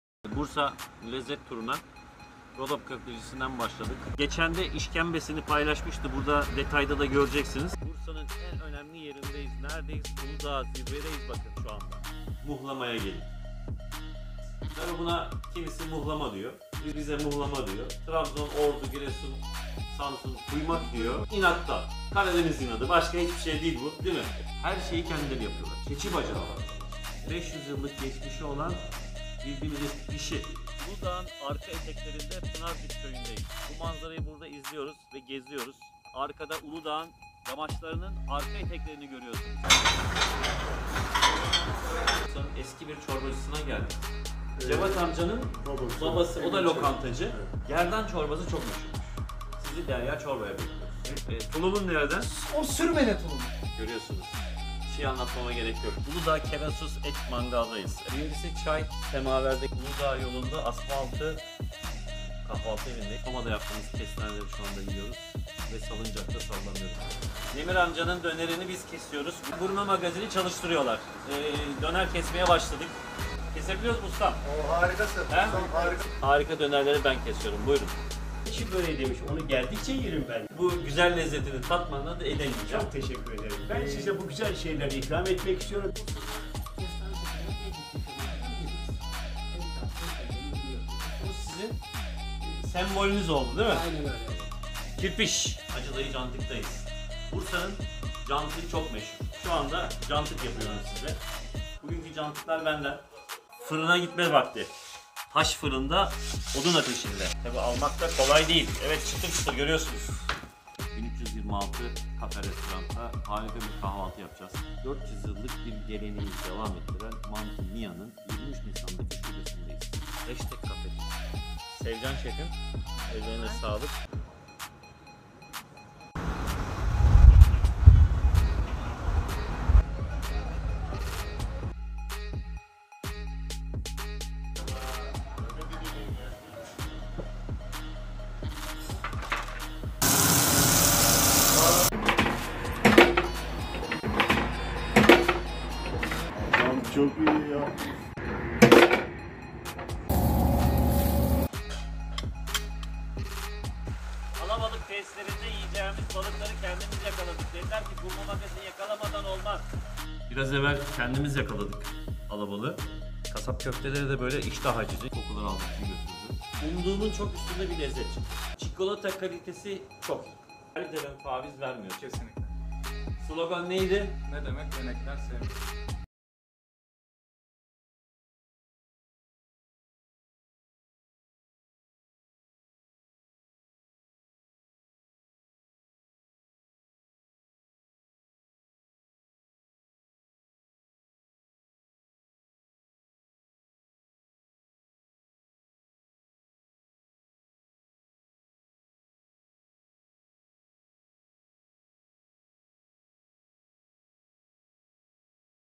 BURSA LEZZET TURU - Mekan Ziyaretleri Çok Neşeli Geçti
Ufuk Güngör'le Gurme Magazin Bursa Lezzet Turu
Bursa'nın Gastronomi anlamında eşsiz lezzetlerini ziyaret ettim. Bu ziyaretimde konaklama ve ulaşım desteği veren @endavet.com a teşekkür ederim.
Rodop Köfte İlker Boy
Mantı Mia 23 Nisan Şubesi
Pınarcık Alabalık Tesisleri
Kerasus Et Mangal
Asmaaltı Kahvaltı Evi
Kardelen Kestane Şekeri
Akay Çiğ Börek
Demir Amca Döner
Hachtag Cafe
Acı Dayı Cantık
1326 Cafe Restaurant
Derya Çorba
Ciğerci Adem Baba
Cunda Eklercisi
Bursa Tereyağcısı
MY Mollaoğlu
Seyahatimde Bize destek olan a teşekkür ederim